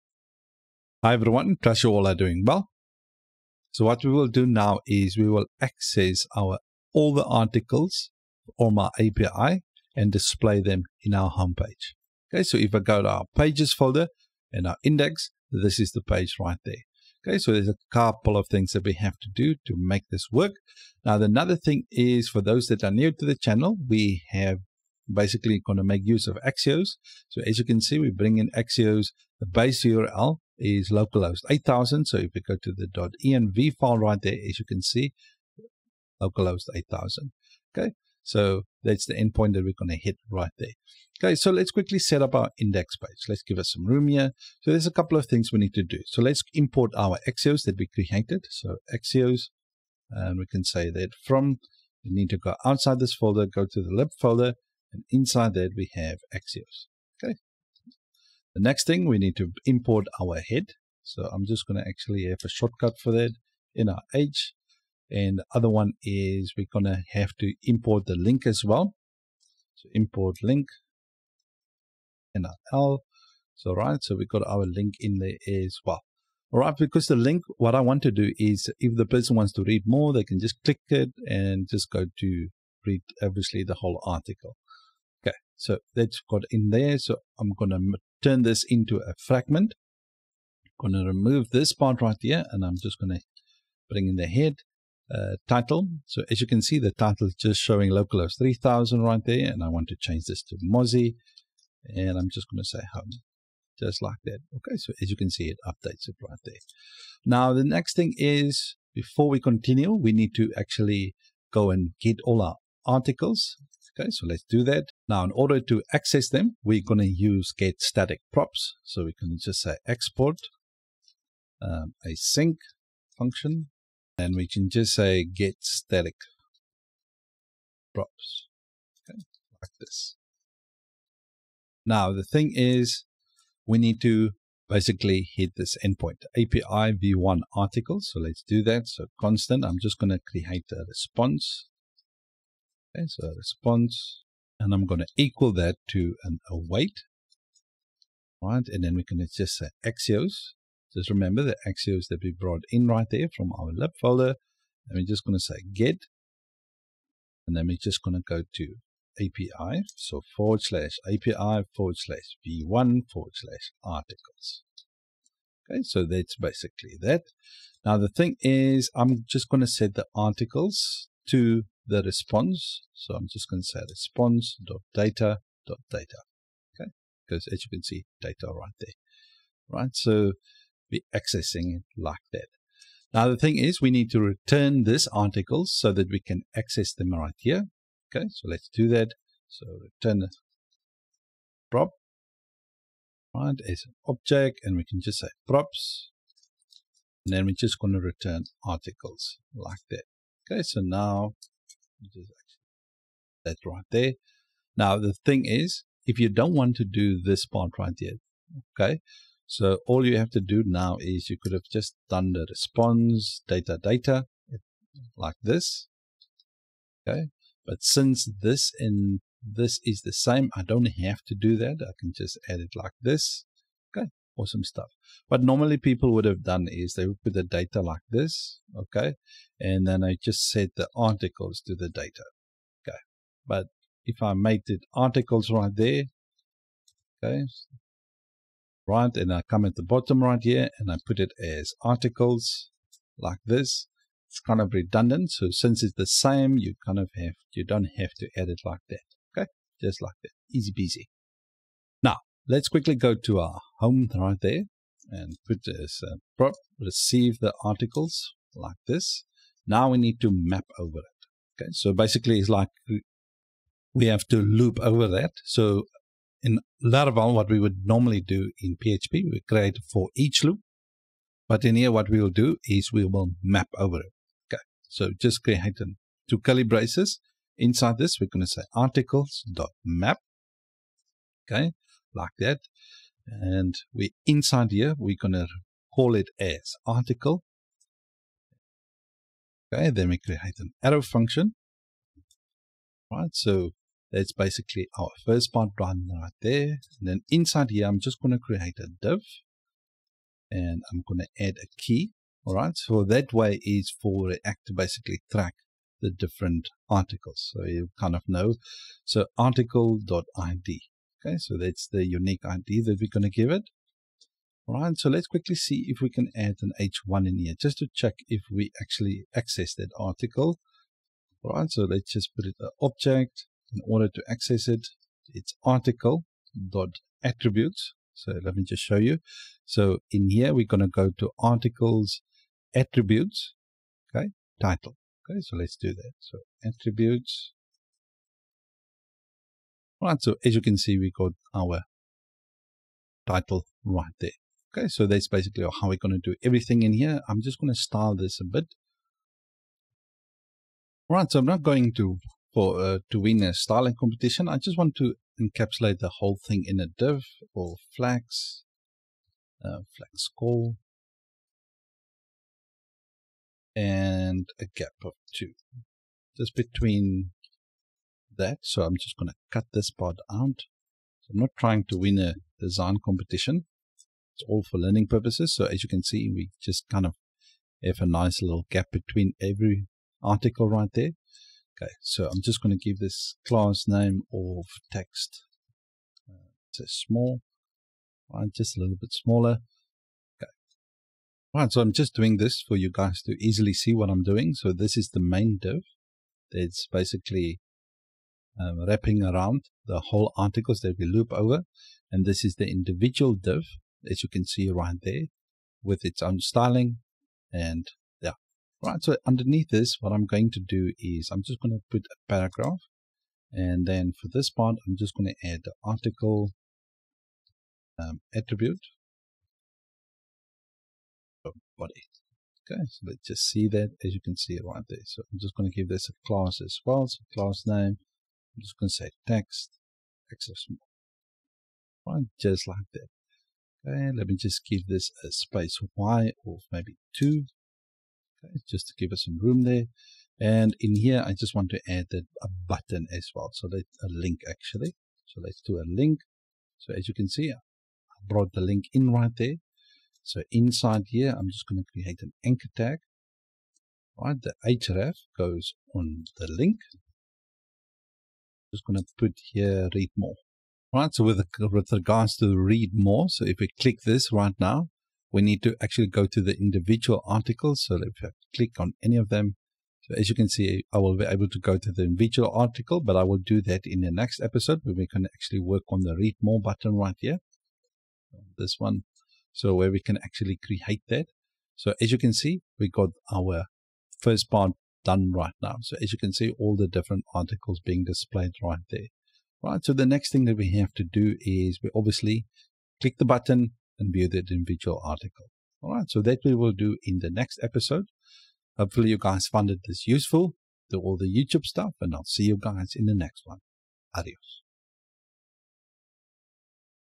Get and display all articles | Laravel REST API and Next.js #16
In this video, we will get and display all articles using getStaticProps.